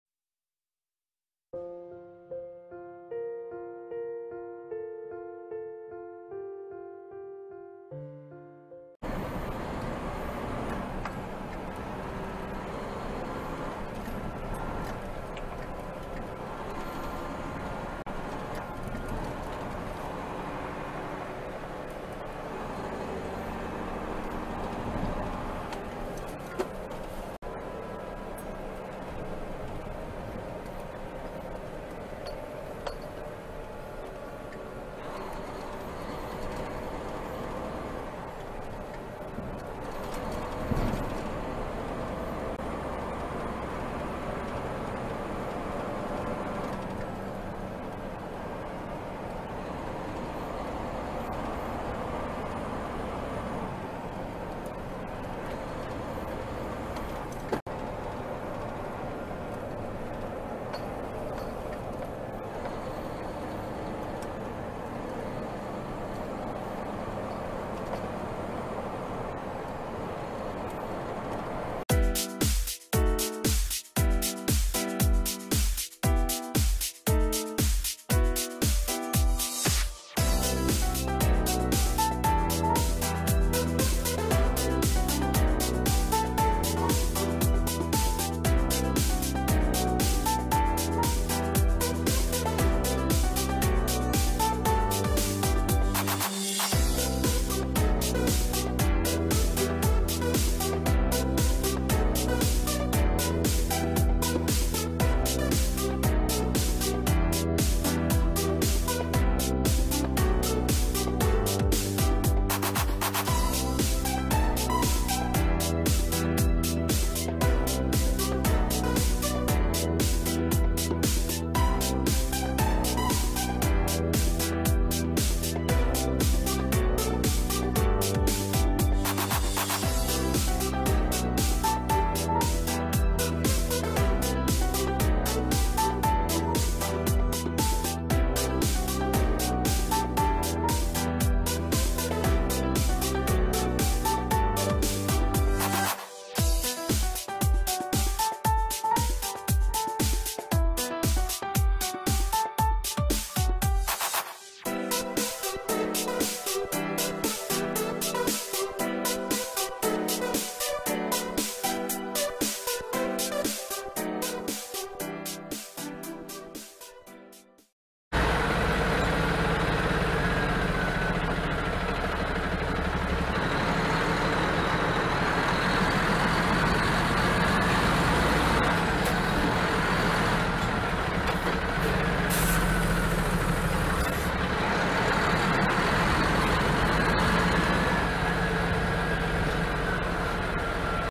迷惑運転者たちNo.276煽りを売る営業車・・【トレーラー】【車載カメラ】
Twitte
１７０℃広角カメラにて撮影しているため

トレーラー
ドライブレコーダー
事故